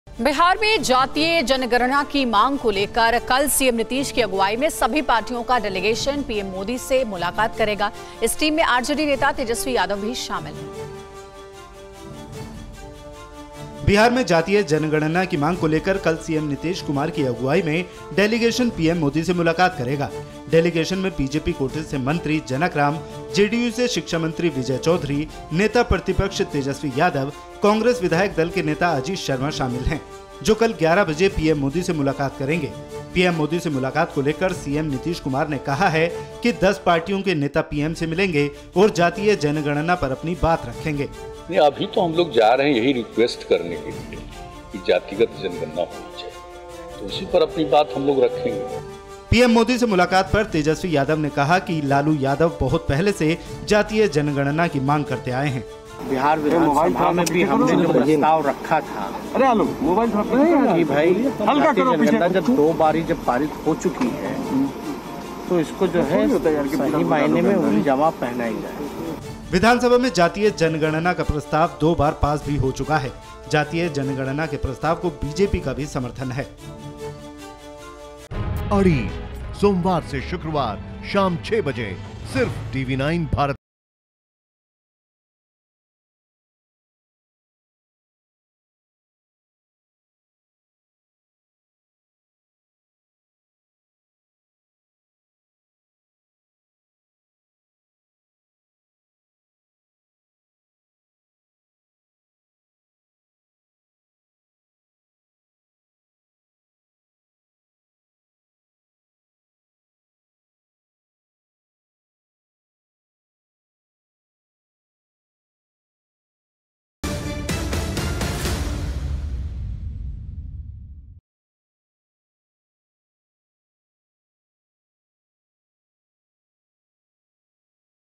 Bihar में जातीय जनगणना की मांग को लेकर कल CM Nitish Kumar की अगुवाई | Caste Based Census | PM Modi
बिहार में जातीय जनगणना की मांग को लेकर कल सीएम नीतीश की अगुवाई में सभी पार्टियों का डेलीगेशन पीएम मोदी से मुलाकात करेगा. इस टीम में आरजेडी नेता तेजस्वी यादव भी शामिल हैं.

Credit